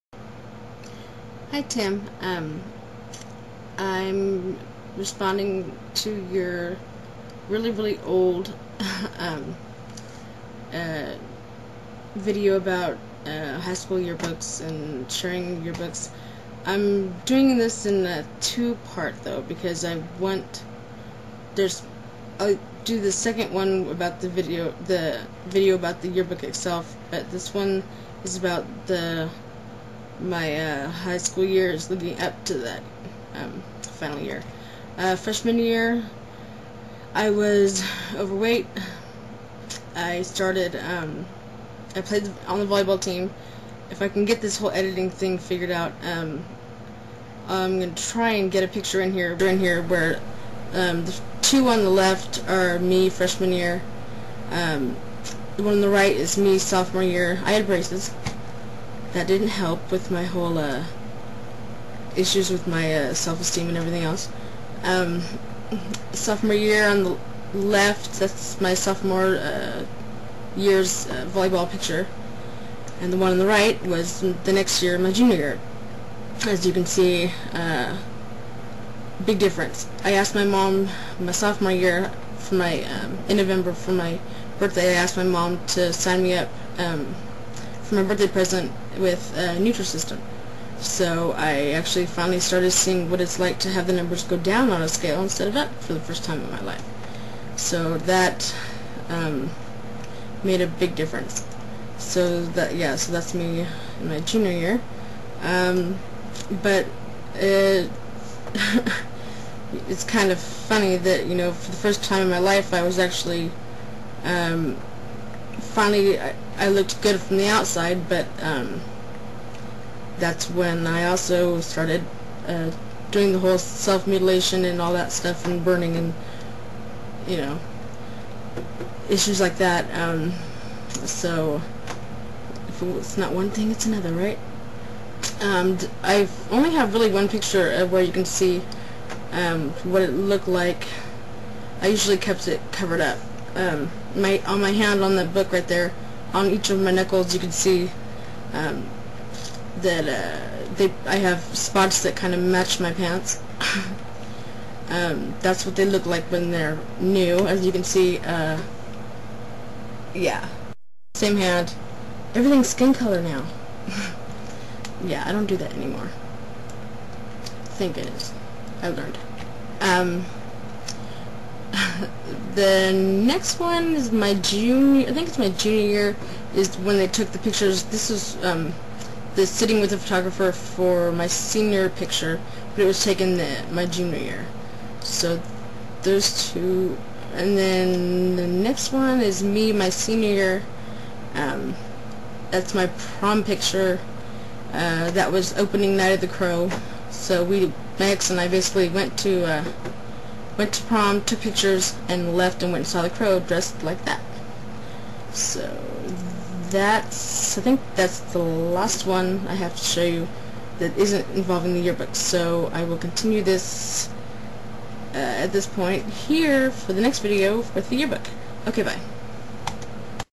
Yearbook vid. #1 (Intro.)
Just a review (via pic.) of my high school years... This is #1 of 2 videos that were going to be used as a response to one of UpDownMostly (Tim)'s videos, which asked for people to share their yearbook... But by the time I found mine, he'd deleted that video... :)